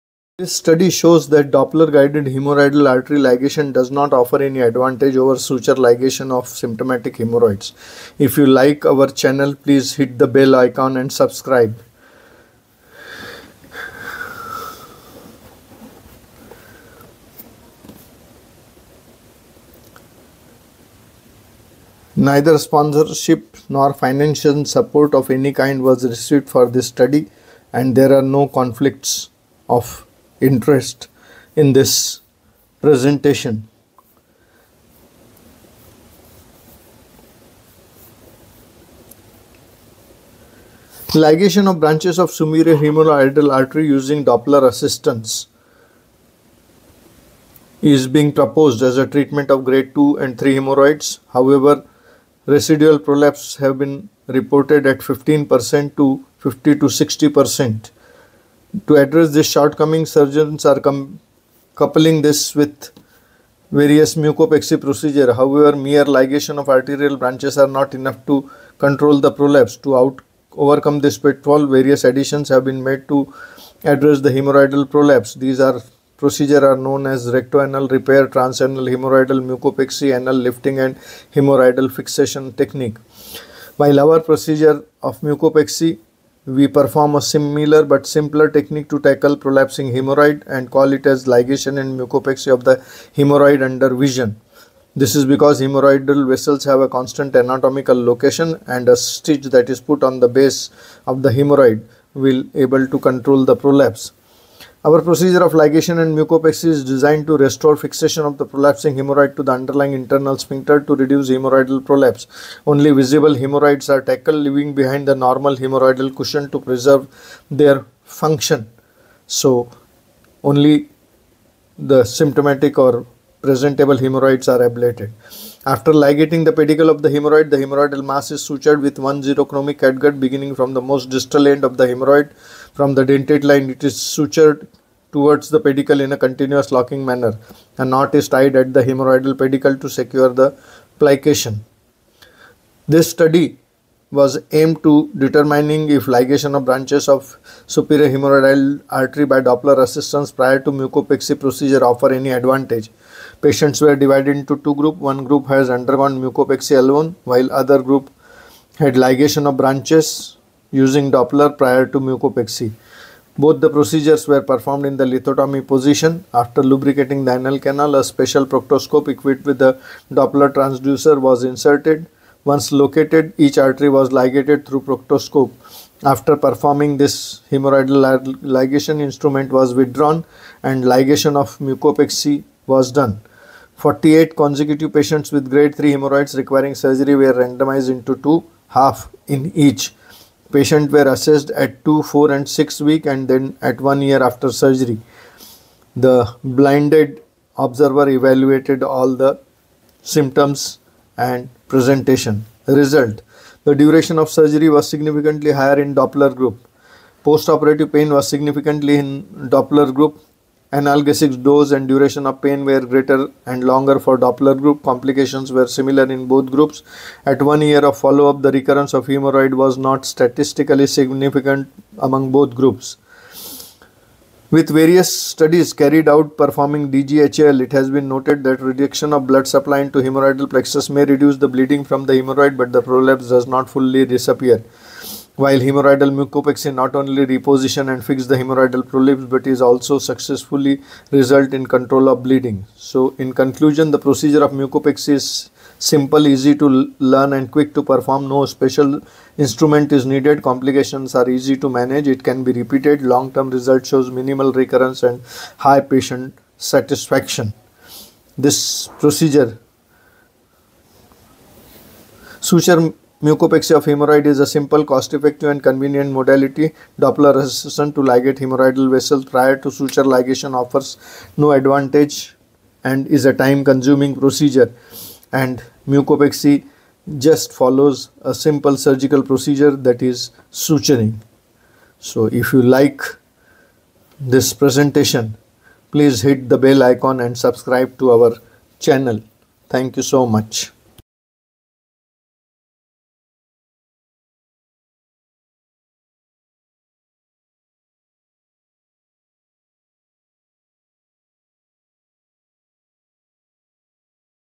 IS DOPPLER ASSISTANCE NECESSARY FOR HEMORRHOIDAL TREATMENT ?
Materials and methods: A double-blind, randomized controlled study was conducted on 48 consecutive patients with grade III hemorrhoids requiring surgery. The patients were randomized into two groups. Half of them were treated with ligation and mucopexy , while the remaining patients underwent a Doppler-guided hemorrhoidal artery ligation followed by ligation and mucopexy . The patients were examined by a blinded independent observer at 2, 4, and 6 weeks and at the end of 1 year after the operation to evaluate postoperative pain scores, amount of analgesics consumed, and complications encountered. The observer also assessed recurrence of hemorrhoids after 1 year.

Conclusions: Suture ligation of hemorrhoids is a simple, cost-effective, and convenient modality for treating grade 3 hemorrhoids. Doppler assistance in ligating the hemorrhoidal vessels prior to hemorrhoidal mucopexy offers no advantage and is a time-consuming procedure.